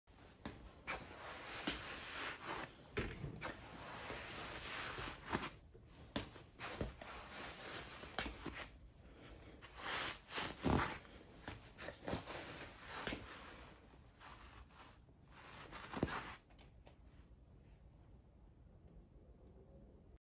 Lint roller
Using a lint roller with my hemi hand.